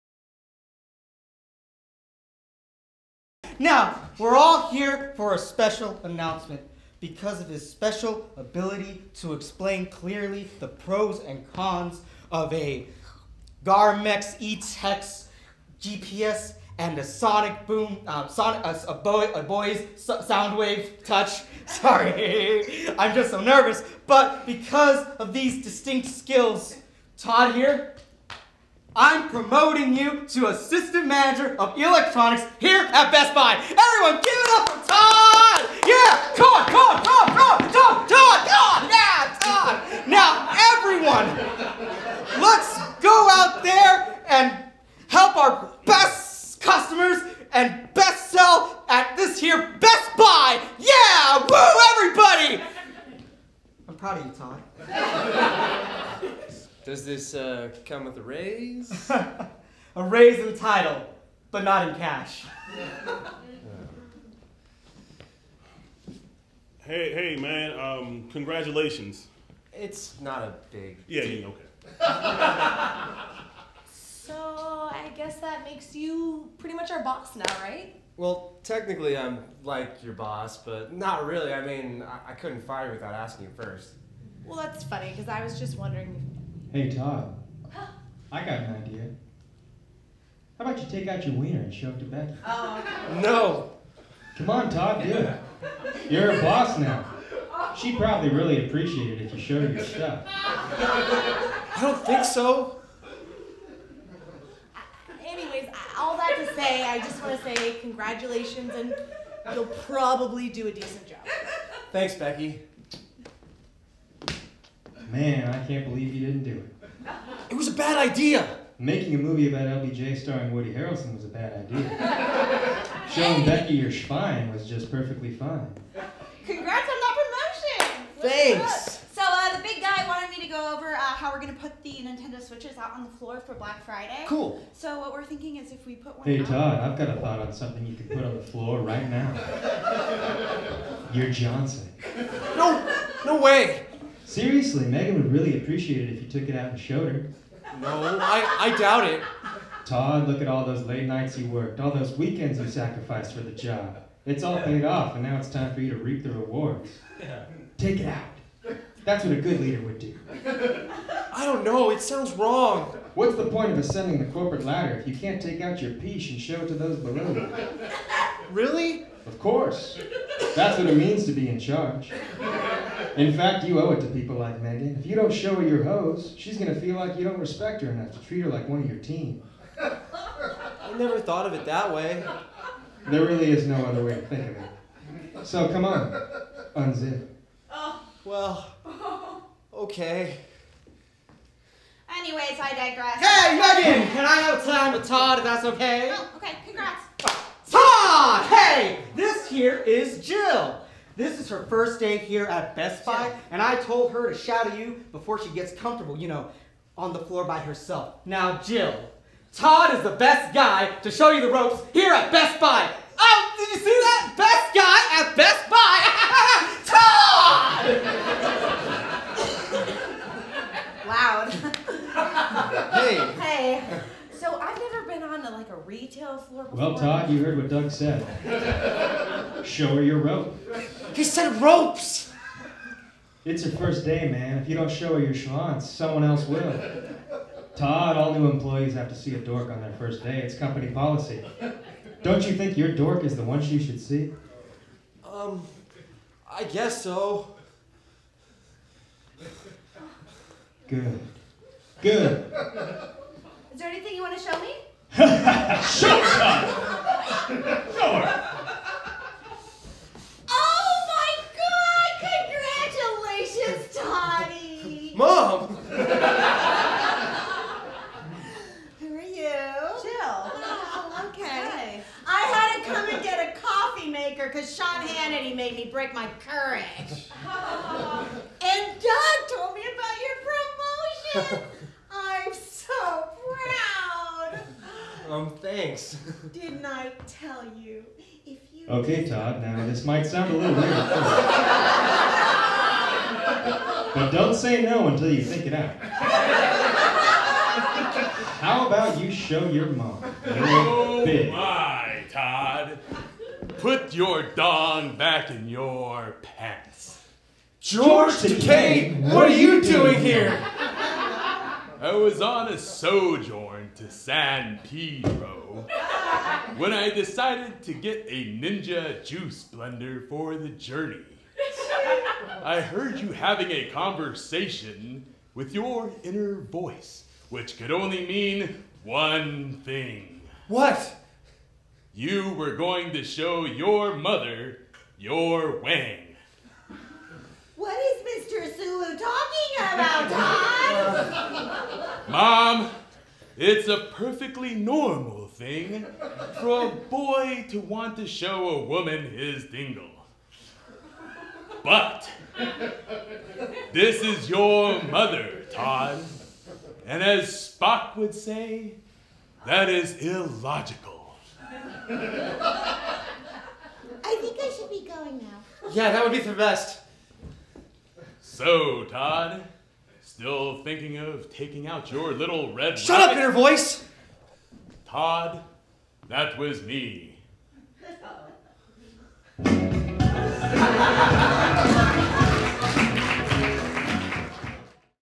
Power
A recent Forbes​ article explained why so many powerful men like Harvey Weinstein, Brett Ratner and Charlie Rose think it's acceptable to expose themselves. Well, what happens when Todd become the new floor manager at Best Buy?

This sketch was written by Peter Aiello and directed by Bill Kelly. It features JP Siruno, Kevin Alain, Jen Kosta, Kenny North, Shelby Monahan, Margaux Hamilton, Tony Kim and Lidia Porto from the Epix series, Get Shorty. It was performed in the November 19th  show, before a live audience, at the Acme Comedy Theatre in Hollywood.